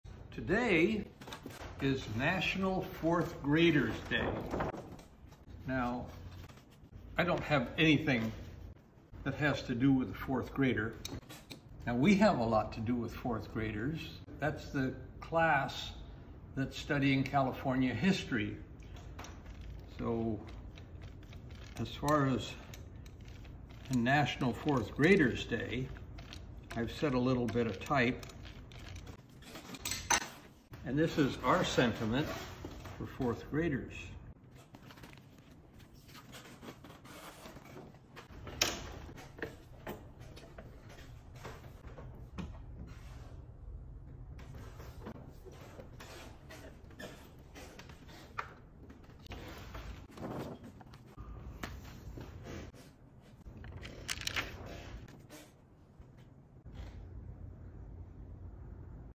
Most of the students that visit us are in the 4th grade because that’s when they study CA history.
Today is National 4th Graders Day! Fourth grade is the grade in which local students learn about California history. Since our museum focuses on early Sacramento history and the California Gold Rush, much of our school programs adhere to 4th grade Social-Science curriculum.

In, honor of today, Howard typeset and printed a phrase in honor of the majority of students that participate in our school programs.

ATTENTION TEACHERS: We have in-person, virtual, and outreach school programs available and we are taking reservations for this school year!